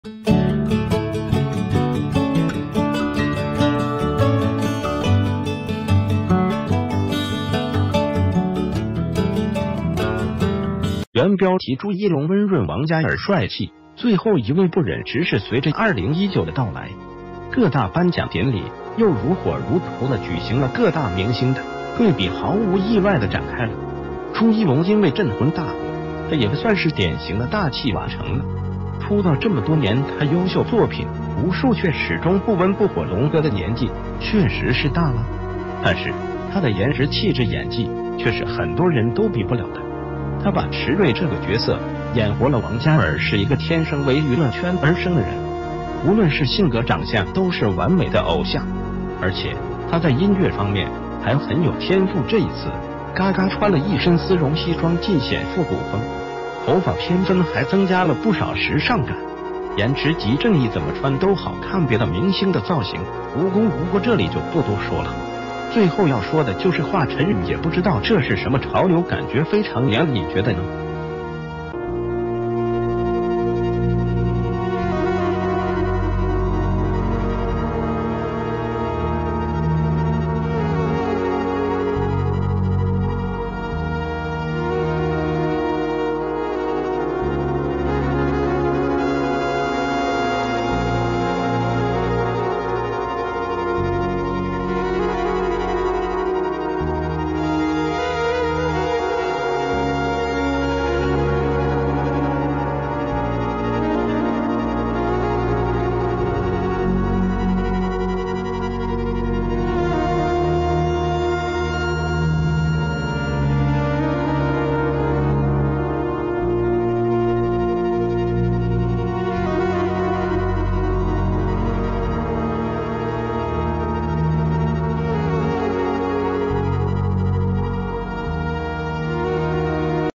朱一龍溫潤王嘉爾帥氣，最后一位不忍直視
谢谢！

      原標題：朱一龍溫潤王嘉爾帥氣，最后一位不忍直視
            隨著2019的到來，各大頒獎典禮又如火如荼地舉行了，各大明星的對比毫無意外的展開了！
朱一龍因為《鎮魂》大火，他也算是典型的大器晚成了，出道這么多年他優秀作品無數卻始終不溫不火

龍哥的年紀確實是大了，但是他的顏值氣質演技卻是很多人都比不了的，他把遲瑞這個角色演活了

王嘉爾是一個天生為娛樂圈而生的人，無論是性格長相都是完美的偶像，而且他在音樂方面還很有天賦

這一次嘎嘎穿了一身絲絨西裝盡顯復古風，頭發偏分還增加了不少時尚感，顏值即正義怎么穿都好看

別的明星的造型無功無過這里就不多說了，最后要說的就是華晨宇，也不知道這是什么潮流，感覺非常娘，你覺得呢